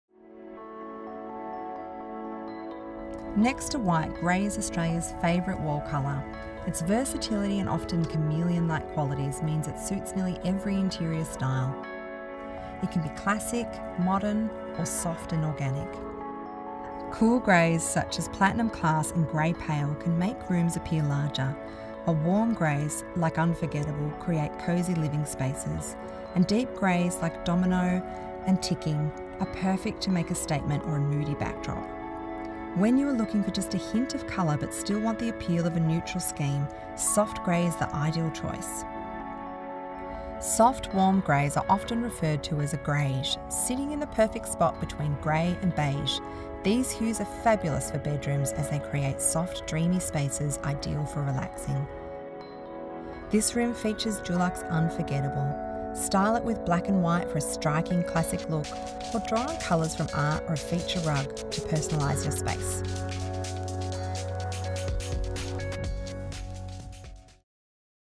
How to use grey paint in your home | Dulux
After white, grey is the next most popular wall colour. Bree Leech shares top tips on how to use grey paint in your home to create different moods.

Grey’s versatile and chameleon-like quality suit almost any design style. Depending on its undertones, grey can be used to create modern, classic or soft and organic styles. Bree shares her top tips on how to transform your home with grey paint, including which hues are best suited to each style and how to finish your space using accessories to complete your look.